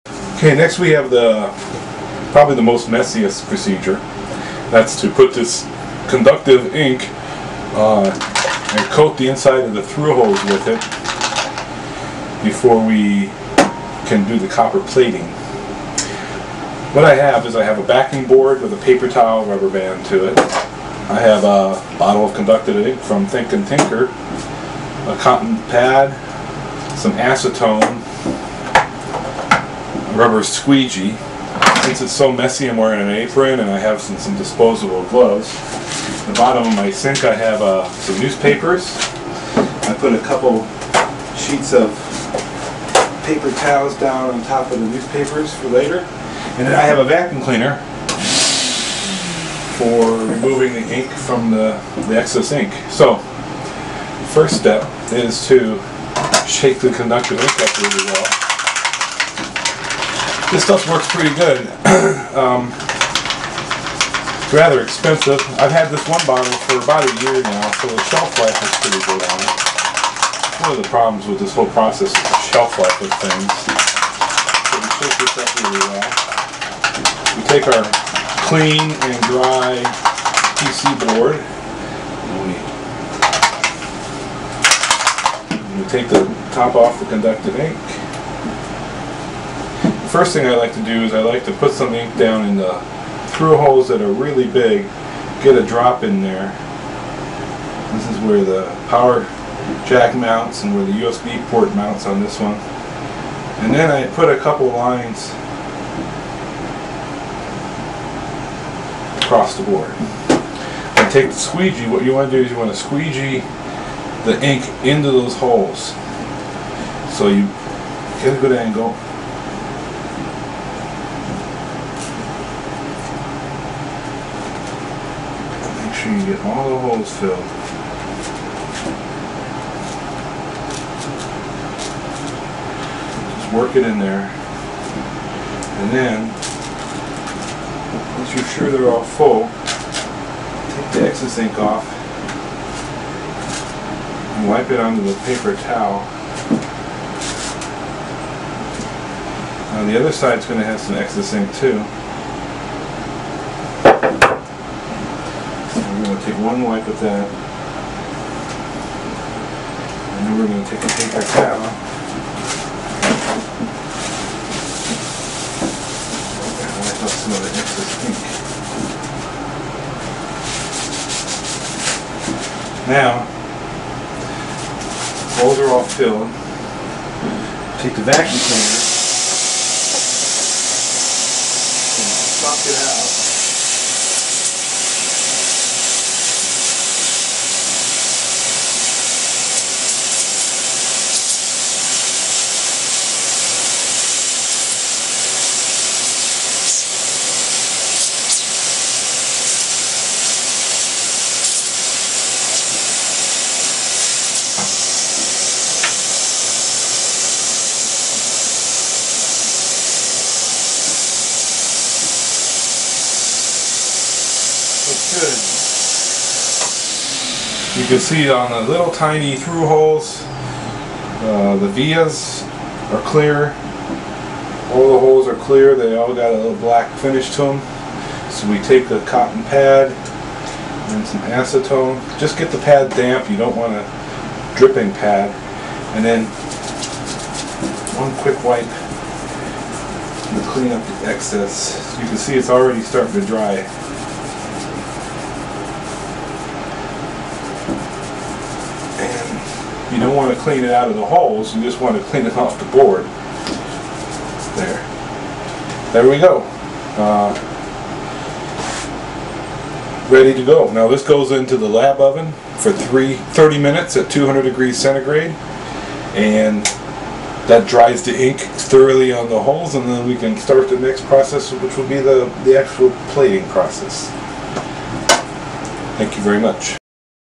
Applying the conductive ink
Applying conductive ink before through hole plating